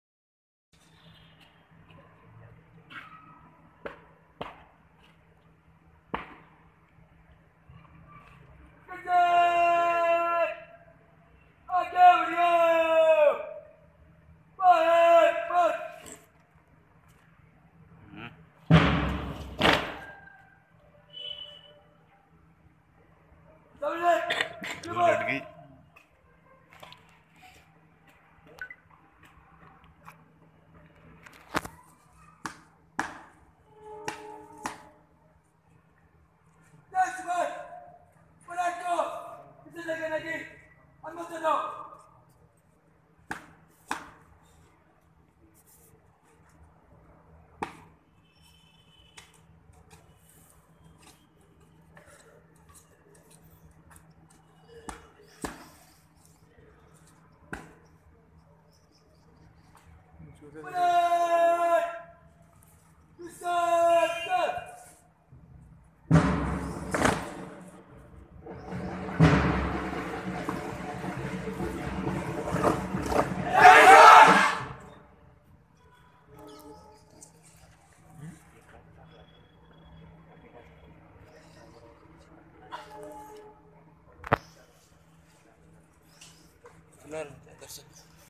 26 janaury 2018 parade video of Badruka college of commerce and arts, Kachiguda, Hyderabad पार्ट 1
Ncc march past video of Badruka college of commerce and arts,Kachiguda,Hyderabad (Telangana)
video create by- smt. sushma devi